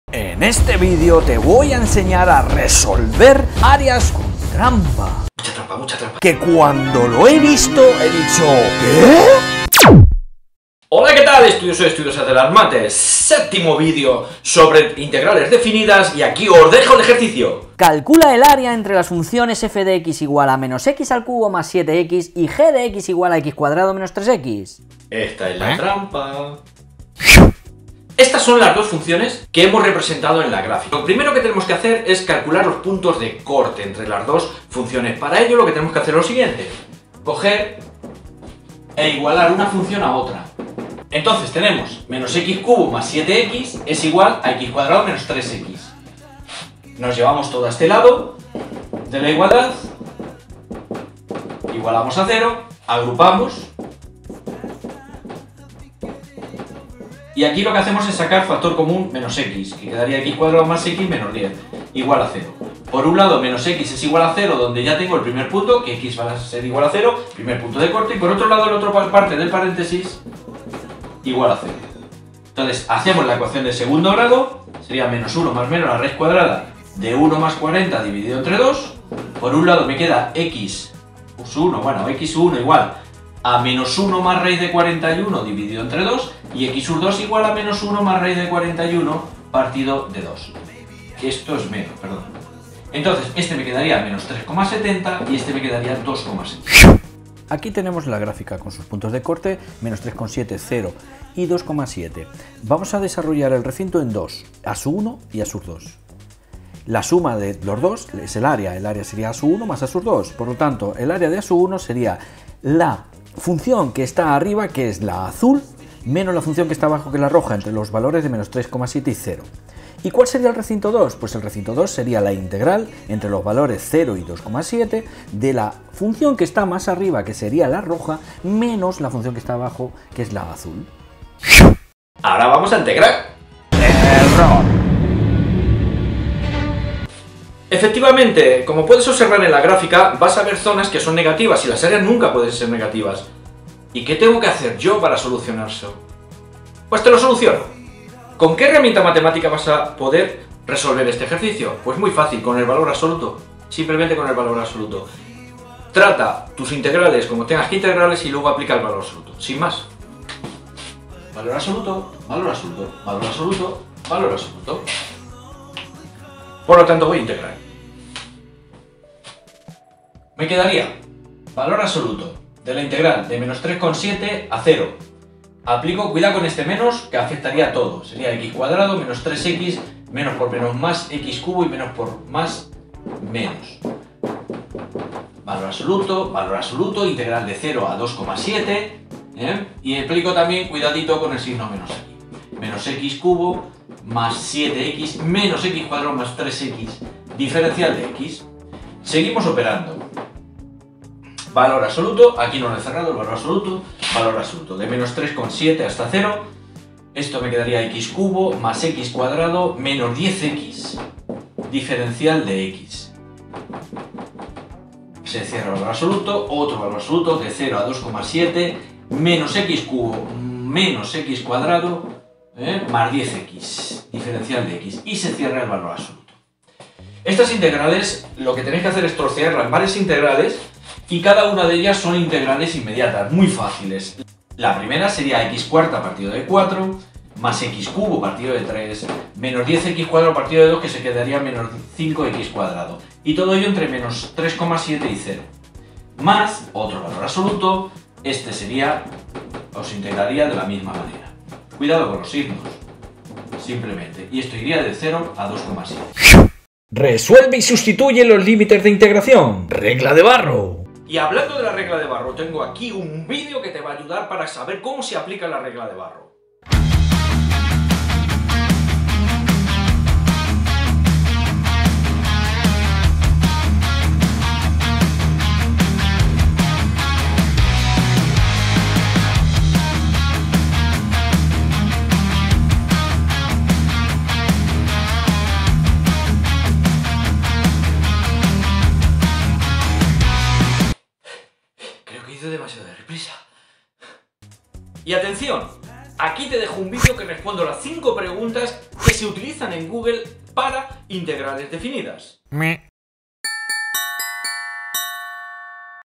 Calcular ÁREA LIMITADA entre FUNCIONES 👉 Área entre Curvas 🧿 INTEGRAL DEFINIDA
📌Recordemos que una integral definida se refiere a un intervalo especifico de una integral, por lo que el proceso se puede resumir de una forma muy simple.
ATENCIÓN! este VÍDEO es FLIPANTE

📱 Calculadora Casio fx-991 ES PLUS